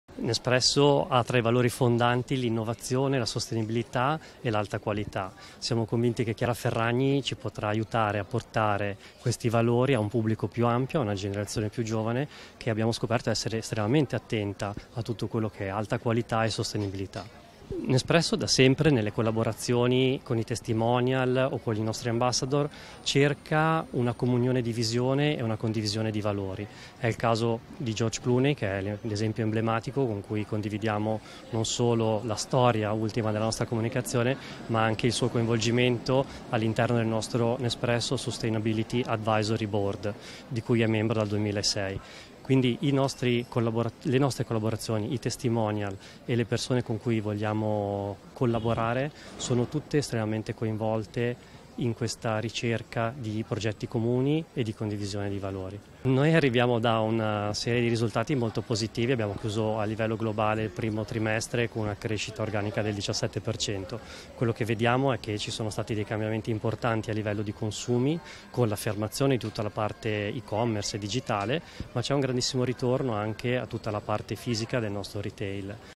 Nespresso, Goglio: "Chiara Ferragni portatrice di nostri valori"
“Nespresso professa da sempre tre valori che sono innovazione sostenibilità e qualità e abbiamo voluto lavorare con Chiara Ferragni perché è in grado di portare questi valori, con trasparenza e semplicità, alle generazioni più giovani, estremamente attente a questi valori; e proprio per la condivisione di questi valori".

Così ha detto Stefano Goglio, direttore generale di Nespresso Italiana, ha spiegato la scelta di Chiara Ferragni nella partnership alla base del nuovo Nespresso X Chiara Ferragni Temporary Cafè, inaugurato a Milano.